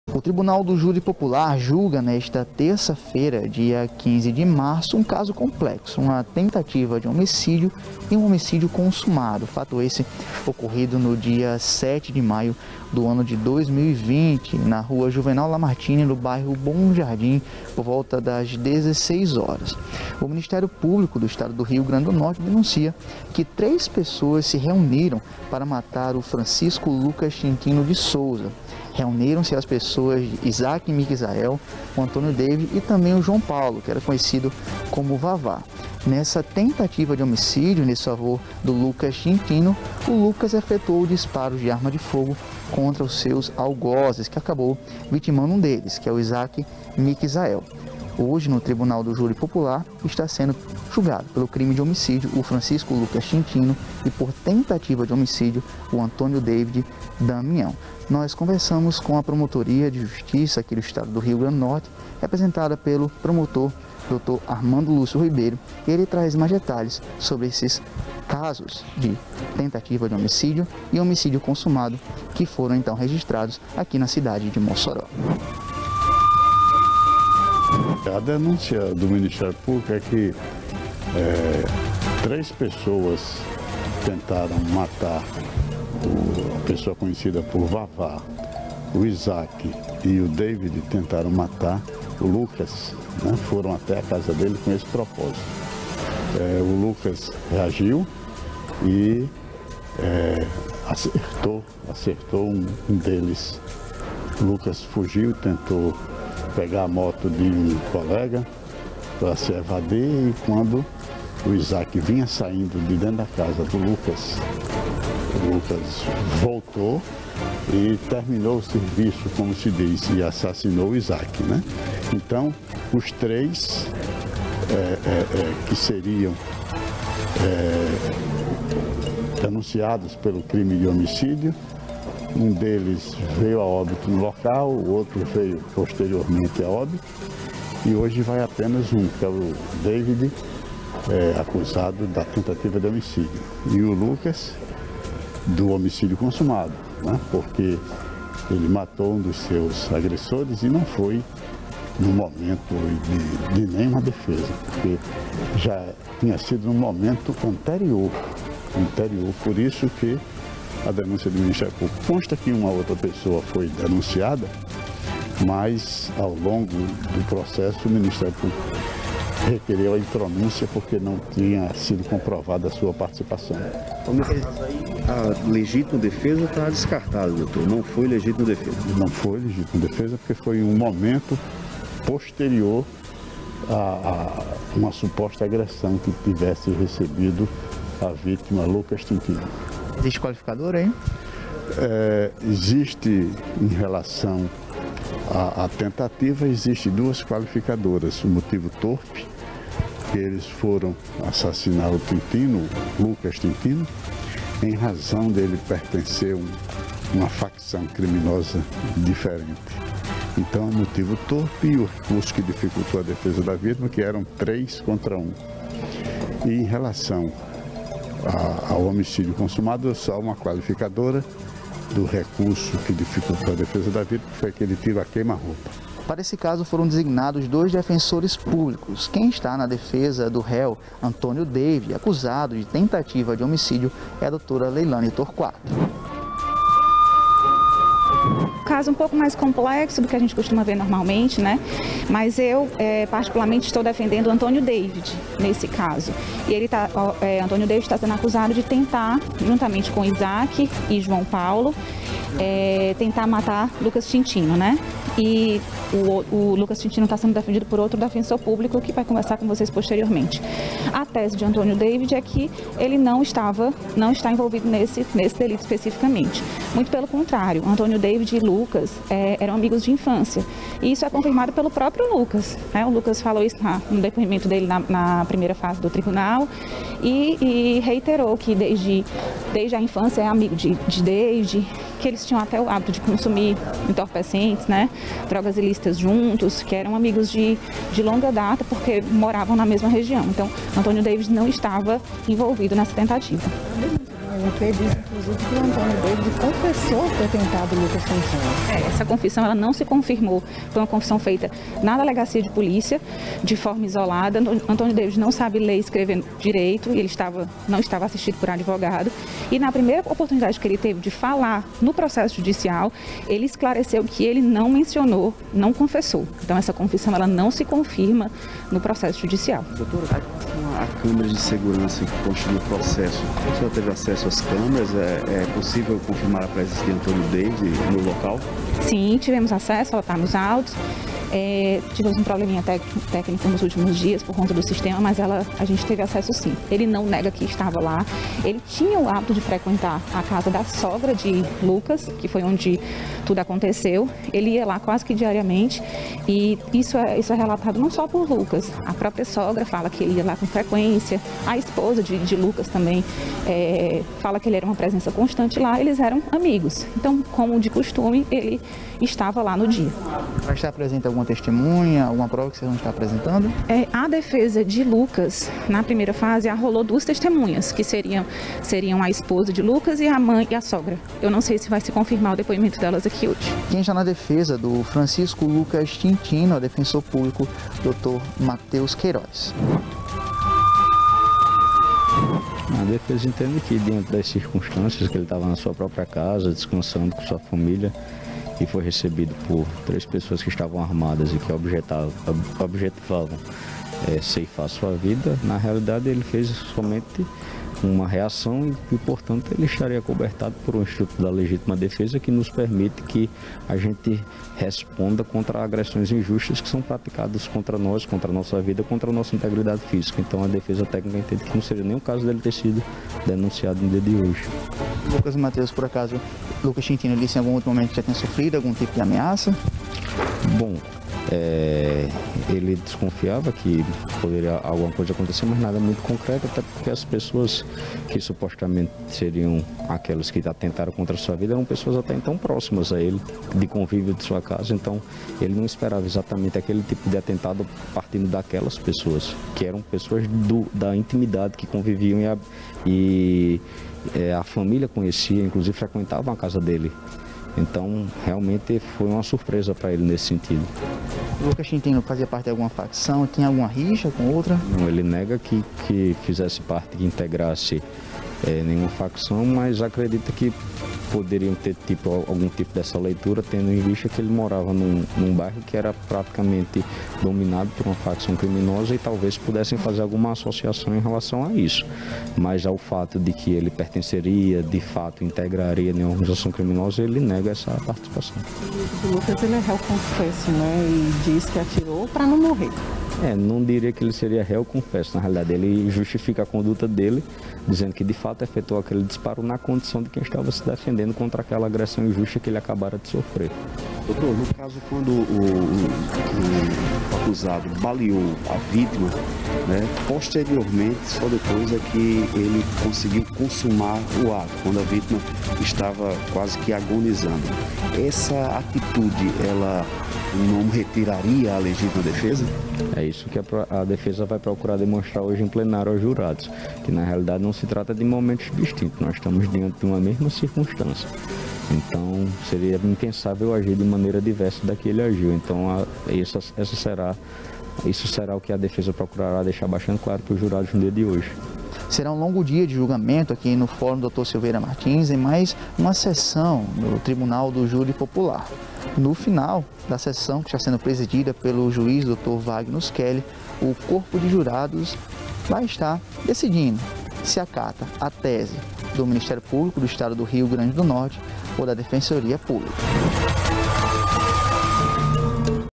TJP JULGA DOIS RÉUS POR HOMICÍDIO E TENTATIVA DE HOMICÍDIO NESTA TERÇA EM MOSSORÓ/RN - 15/03/2022
TJP JULGA DOIS RÉUS POR HOMICÍDIO E TENTATIVA DE HOMICÍDIO NESTA TERÇA-FEIRA EM MOSSORÓ